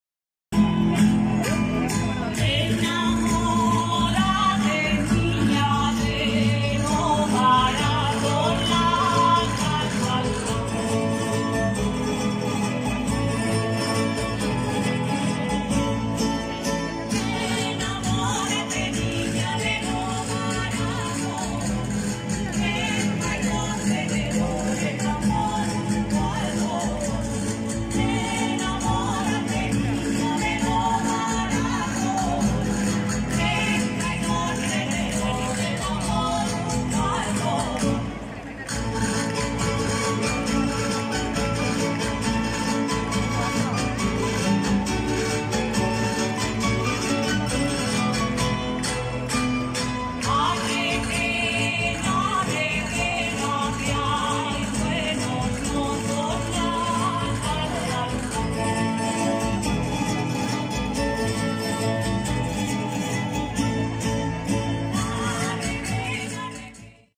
Balls al carrer 2021 - InfosValencia
Todos los domingos y festivos hasta el 19 de diciembre a las 10.30 empieza el espectáculo. En 2022 se retoman el 13 y 20 de febrero, 3 y 10 de abril, y el 15, 22 y 29 de mayo.
Una ocasión perfecta para conocer de cerca la indumentaria tradicional y la música de los instrumentos típicos de nuestra tierra: la dolçaina y el tabalet.